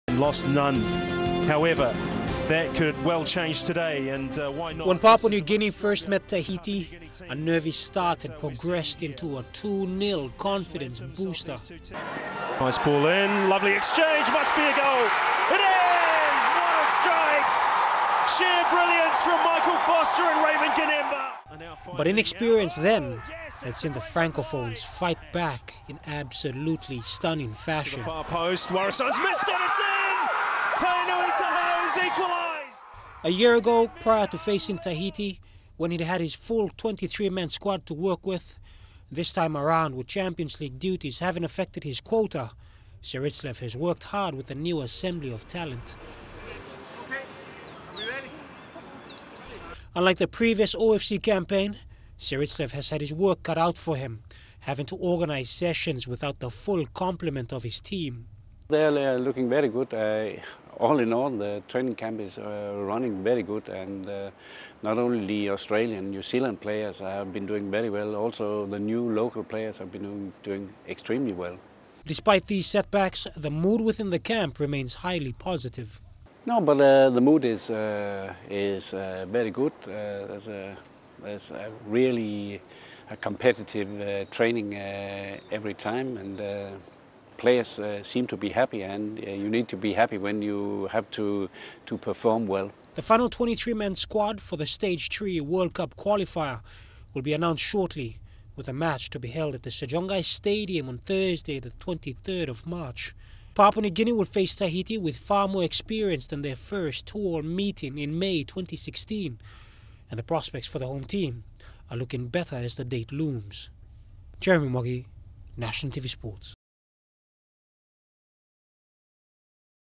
PNG Men’s National Football team doing well
Head coach of the PNG Men’s National team, Flemming Serritslev, has said the squad’s preparations ahead of their match against Tahiti is going well.

With only twelve days remaining until the match, a handful of players are still yet to attend the squad, with striker Raymond Gunemba and Keeper Ronald Warisan on champions league duties.